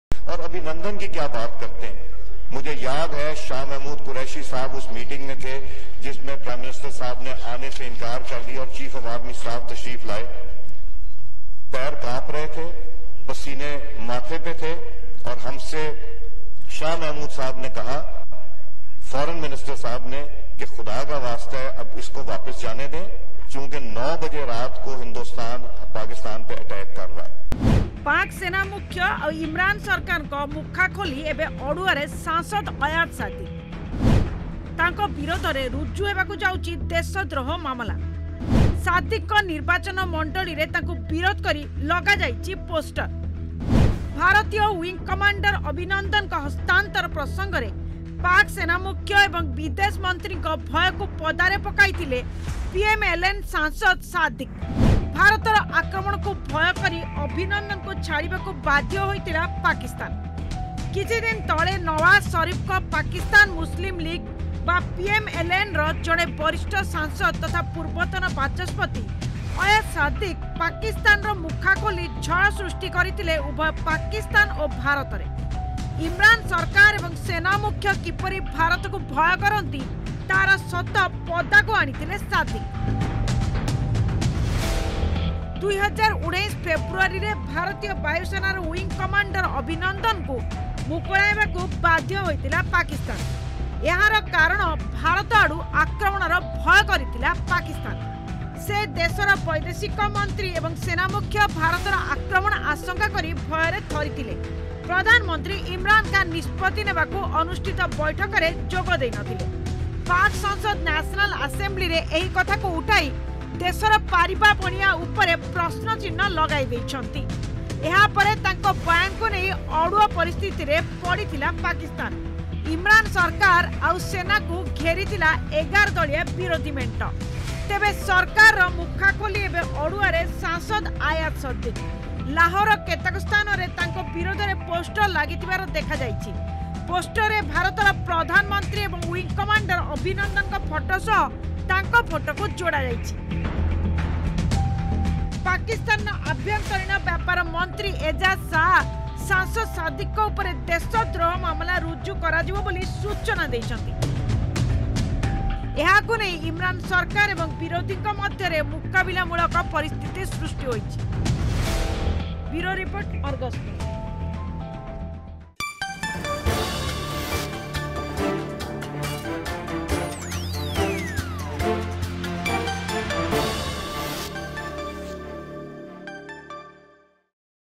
ପାକ୍‌ସେନାର ମୁଖା ଖୋଲି ଅଡ଼ୁଆରେ ସାଂସଦ ସାଦିକ୍‌.
ପାକ୍ ସେନାମୁଖ୍ୟ ଆଉ ଇମ୍ରାନ୍ ସରକାରର ମୁଖା ଖୋଲି ଏବେ ଅଡ଼ୁଆରେ ସାଂସଦ ଅୟାଜ୍ ସାଦିକ୍‌ । ତାଙ୍କ ବିରୋଧରେ ରୁଜ୍ଜୁ ହେବାକୁ ଯାଉଛି ଦେଶଦ୍ରୋହ ମାମଲା...